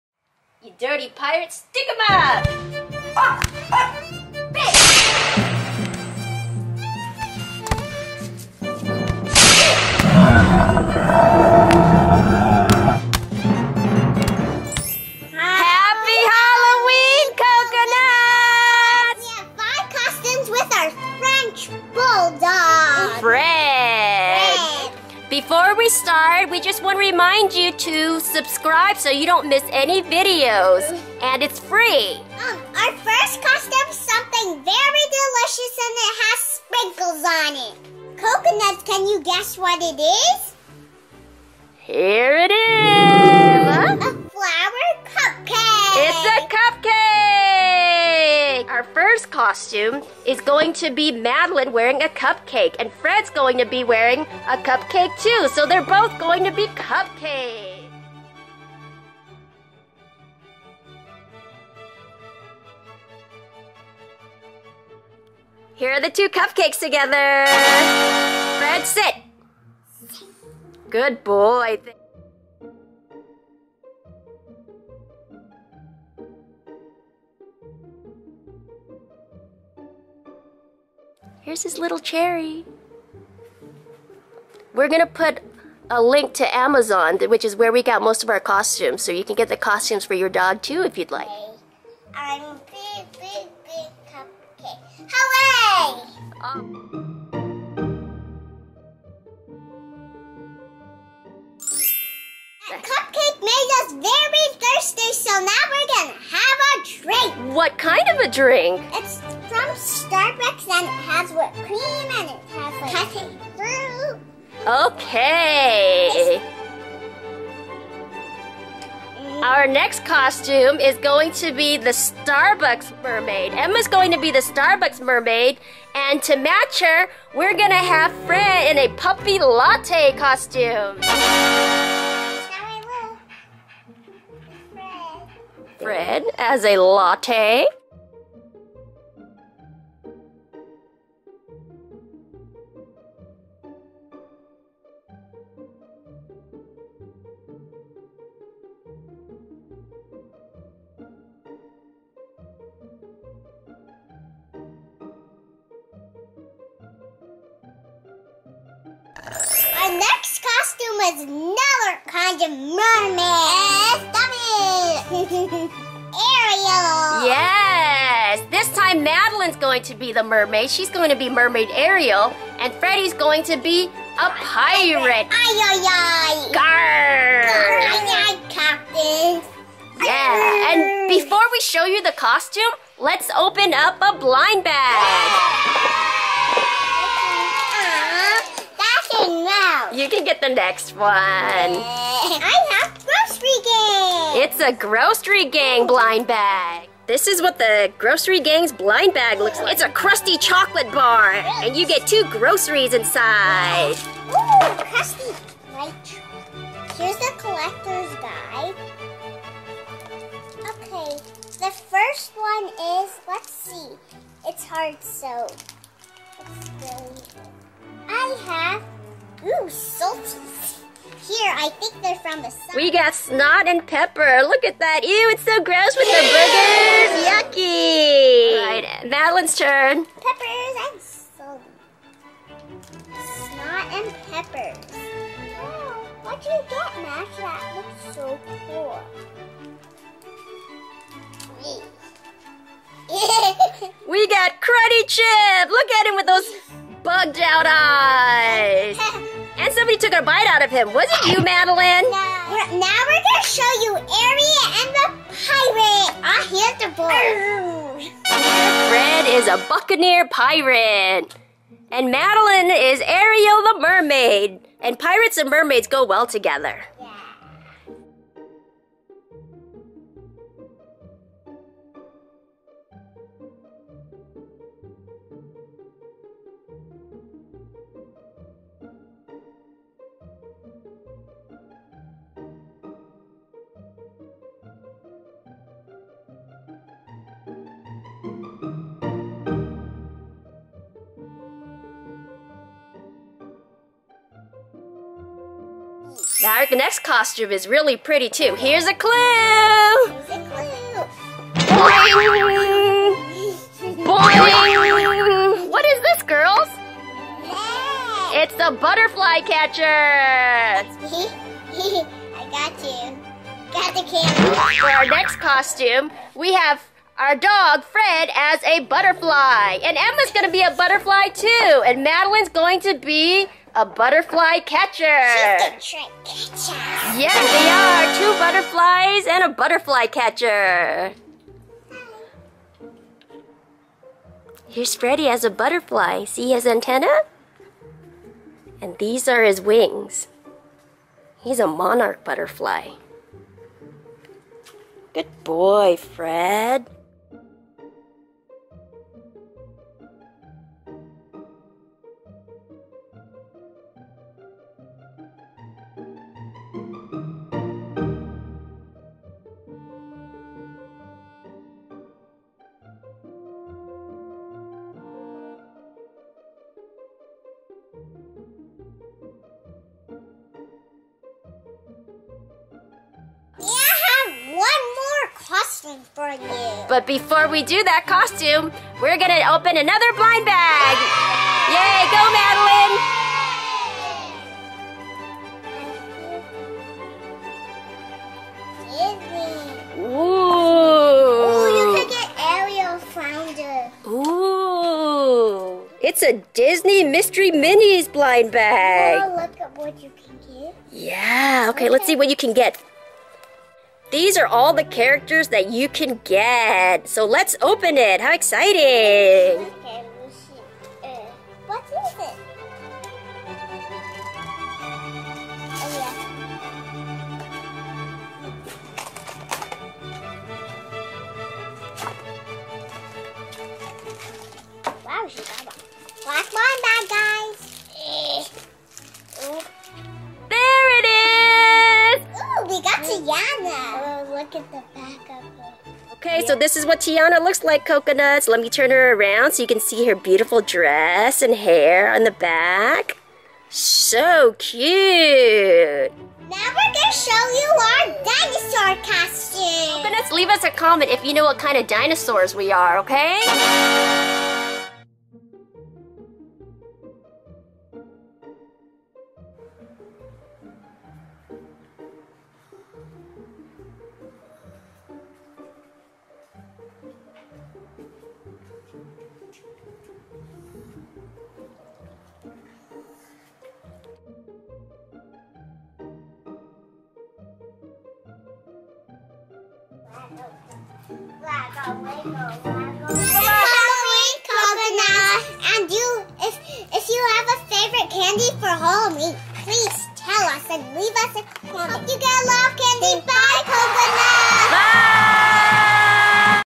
FUNNY French Bulldog Halloween Costumes w/ Matching Kids Costumes! BLIND BAGS I Uh Oh Coco
We dressed up our french bulldog in 5 different Halloween costumes, and we have costumes to match!  The matching costumes are: dog cupcake & kid cupcake, Starbucks mermaid & dog latte/ iced coffee, Ariel Mermaid & dog pirate, dog & kid butterfly and butterfly catcher, dog and kids triceratops dinosaur costume!

Rubie's Cupcake Dog Costume:

Rubie's Dog Puppy Latte Costume:

Animal Planet Dog Butterfly Costume:

Animal Planet Triceratops Dog Costume:

Rubie's Dog Pirate Hat (not ours, but just as cute!):